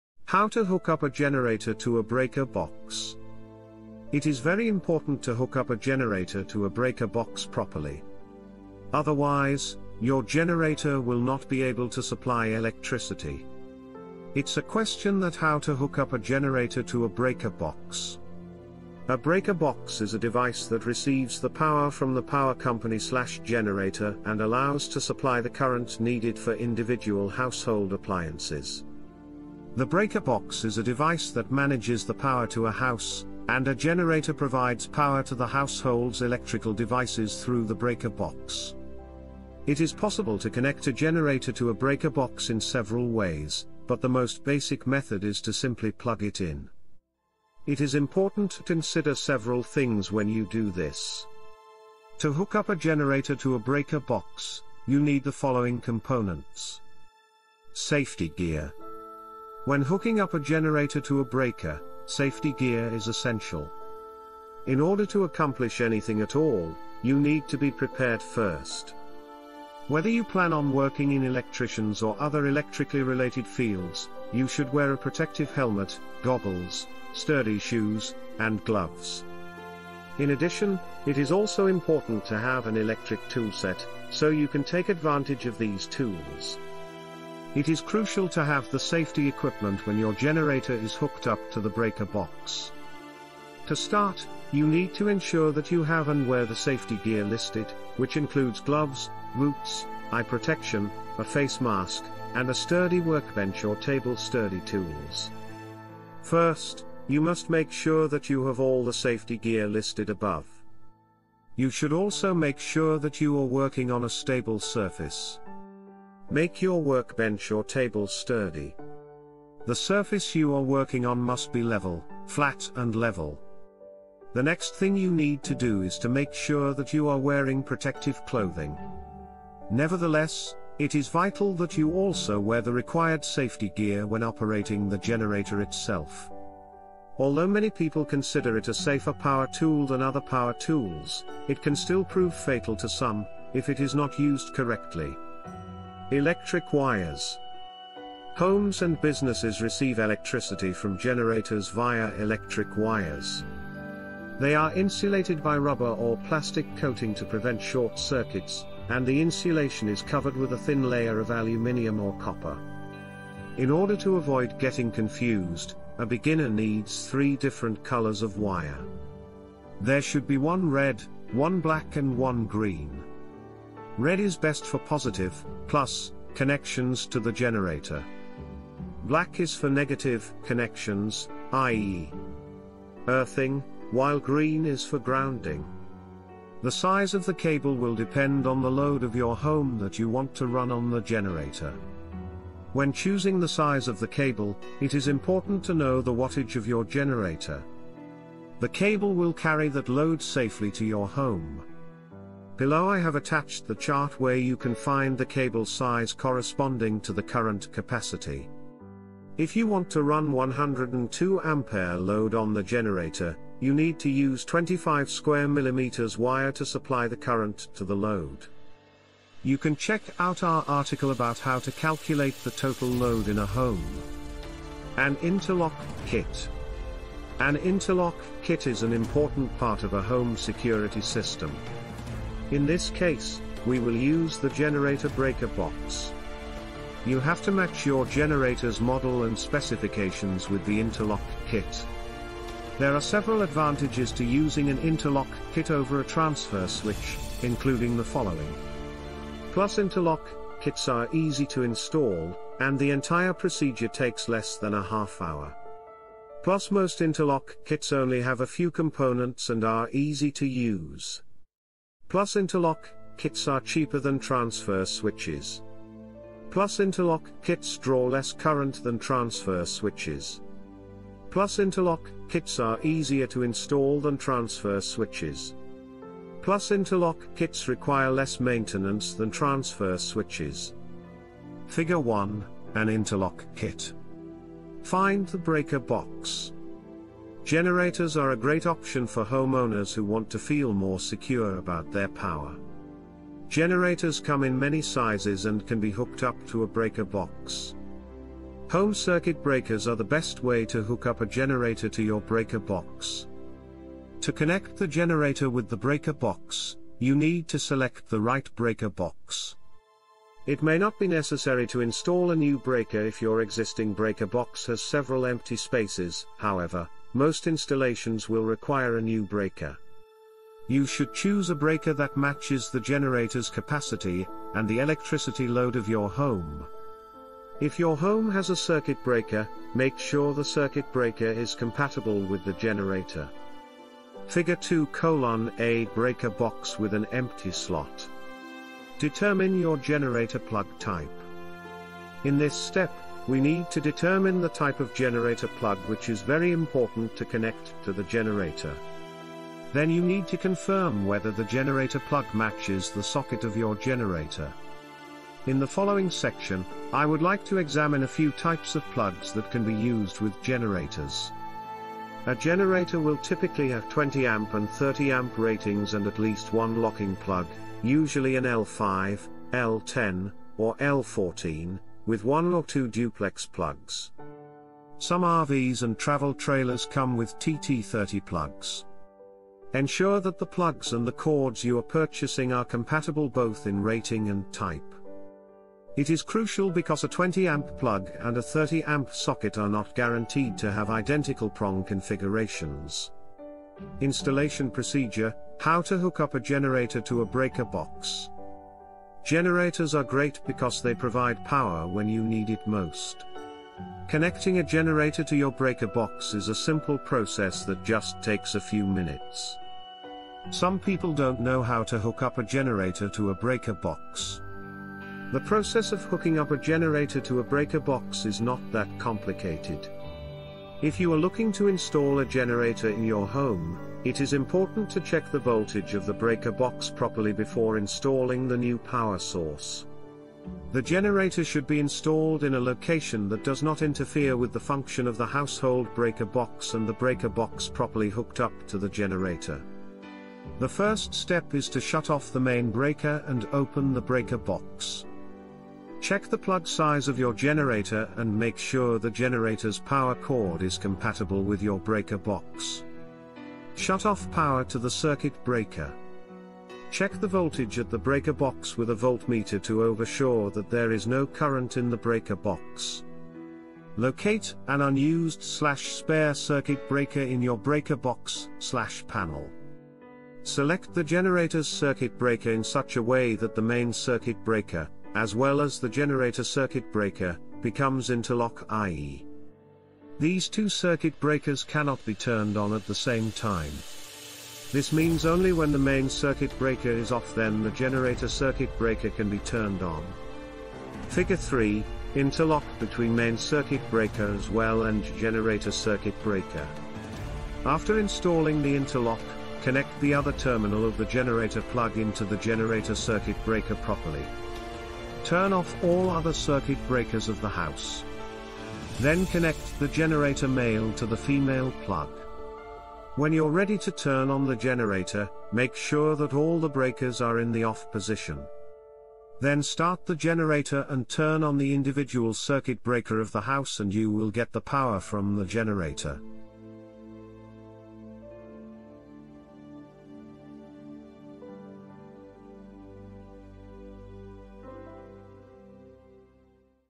How to Hook Up a Generator to a Breaker Box
It is very important to hook up a generator to a breaker box properly. Otherwise, your generator will not be able to supply electricity. It's a question that How to Hook Up a Generator to a Breaker Box? A breaker box is a device that receives the power from the power company/generator and allows to supply the current needed for individual household appliances.